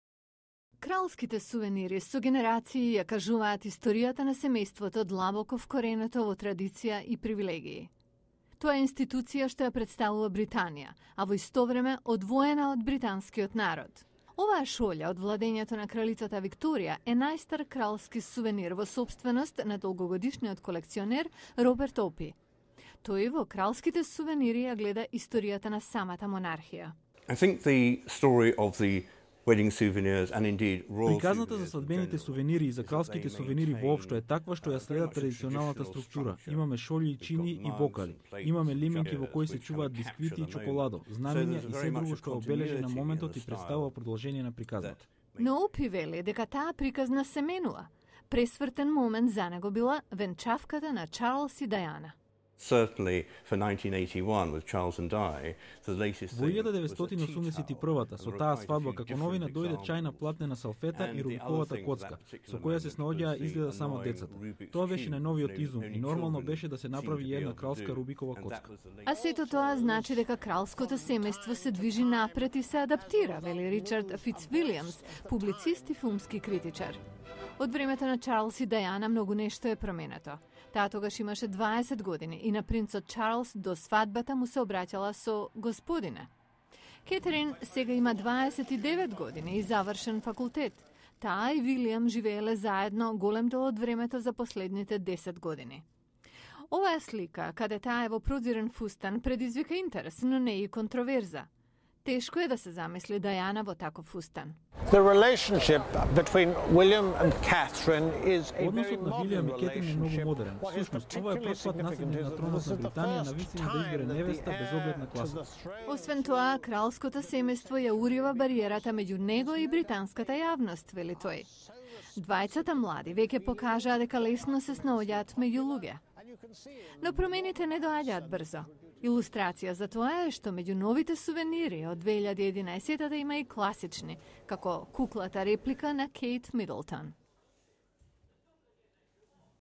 Кралските сувенири -- одраз на новите времиња
Што можат - лимена кутија за кекси, Рубикова коцка и карти за играње, да кажат за британското кралско семејство? Некои велат -- многу. Следниот прилог од Лондон зборува за менувањето на природата на кралските сувенири што е одраз на менувањето на самото кралско семејство.